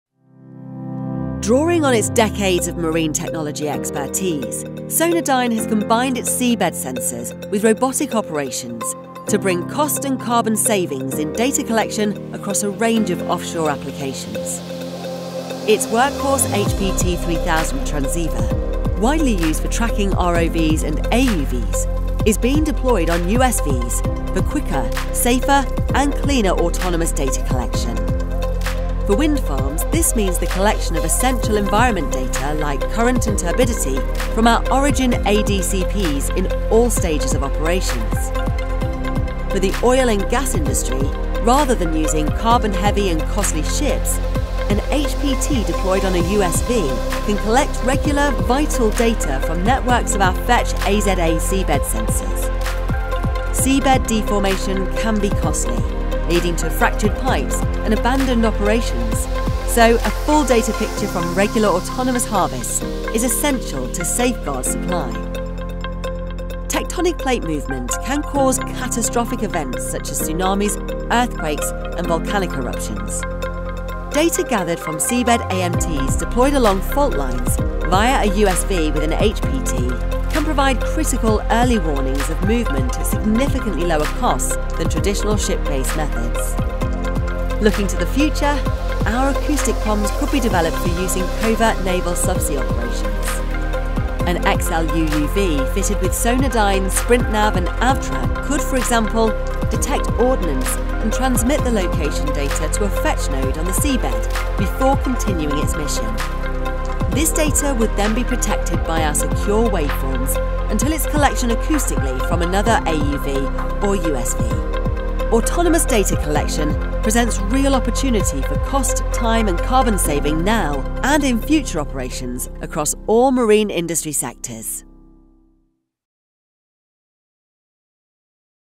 Remote data harvesting - how do we do it?
Working directly with the world’s leading manufacturers of USVs, AUVs and ROVs, we’re able to offer you a complete end-to-end data harvesting service, one where we take care of vehicle selection, logistics, offshore operations and secure data delivery to where it’s needed, when it’s needed.

Timestamps
00:00 - Introduction
00:16 - HPT 3000
00:29 - Wind farm applications
00:40 - Oil and gas applications
01:07 - Tectonic plate movement
01:28 - Defence applications